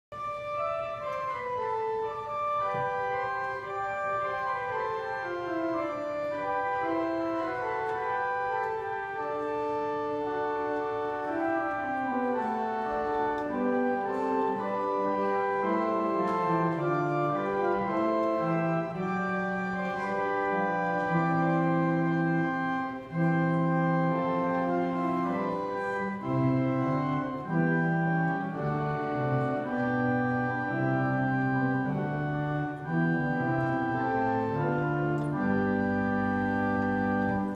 Youth Sunday
Final Practice for youth Sunday at St. Luke's Episcopal North Little Rock